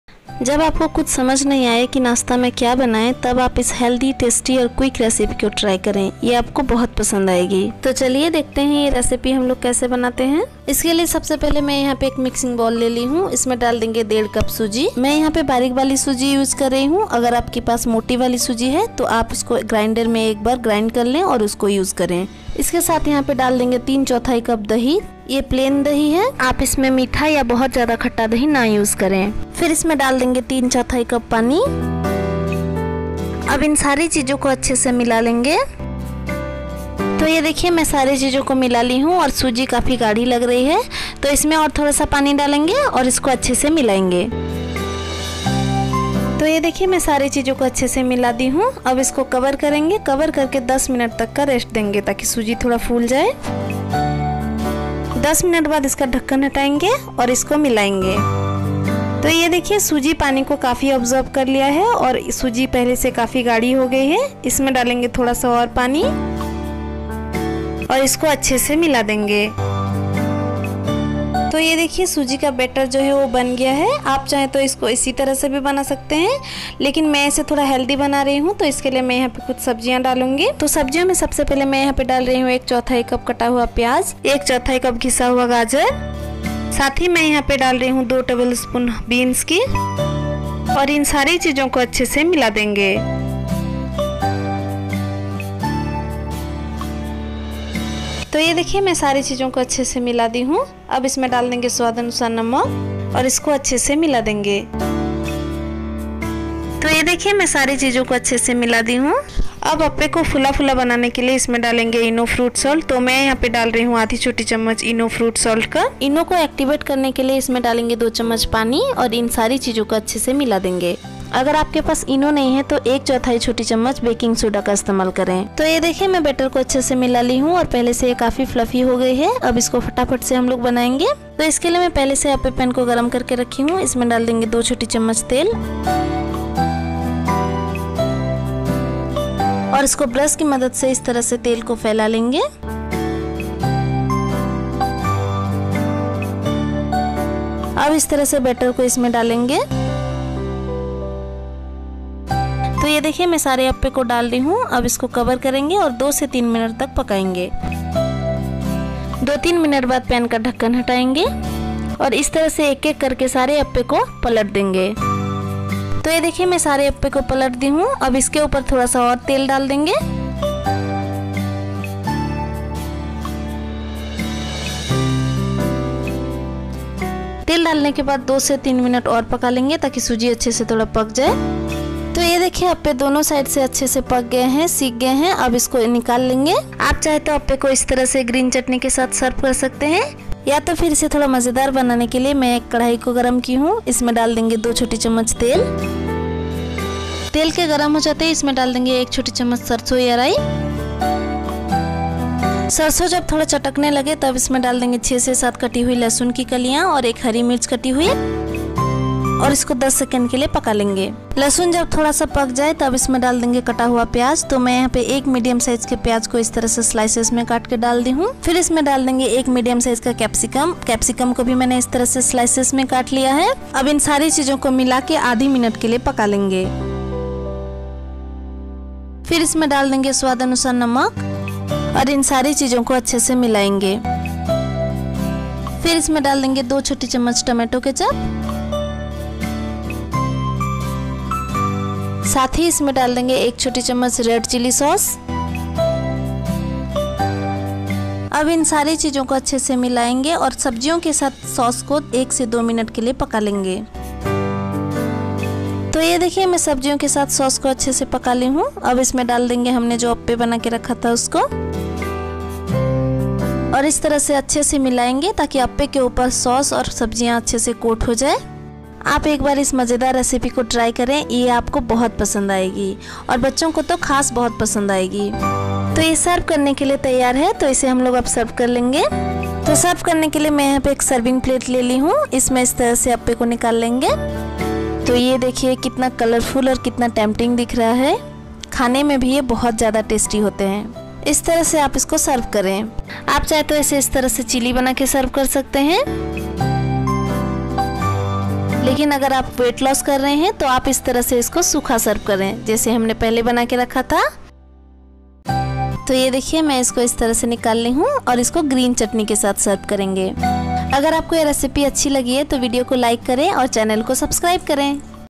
सूजी का हेल्थी टेस्टी और आसान ब्रेकफास्ट रेसिपी | अप्पे चिल्ली रेसिपी | appe chilli recipe
Ingredients -
for appe -

Sujee - 1½ cup
eno fruit salt - 1 tspn

for chilli -
tomato ketchup - 2 tspn
red chilli sauce - 1 tspn